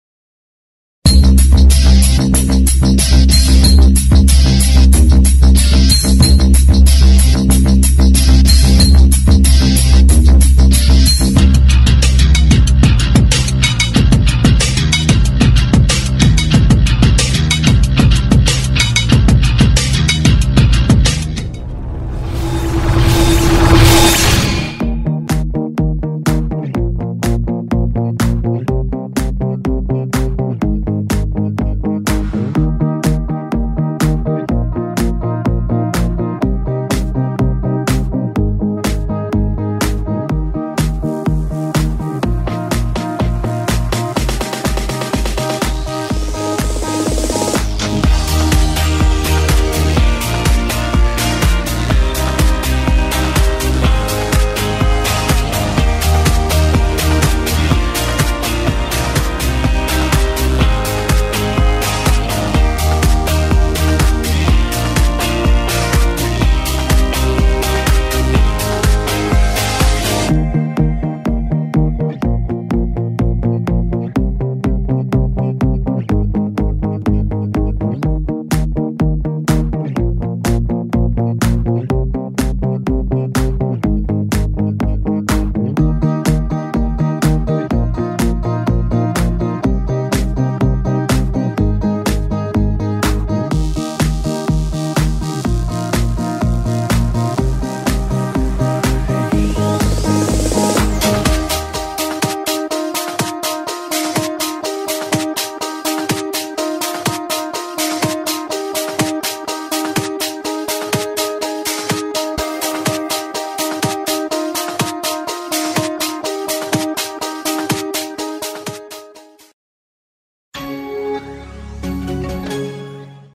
Intel® NUC As a Consumer PC: Big Performance, Small Footprint
Use your Intel NUC for home entertainment, media production, immersive gaming or to save space in your home office. The possibilities are endless.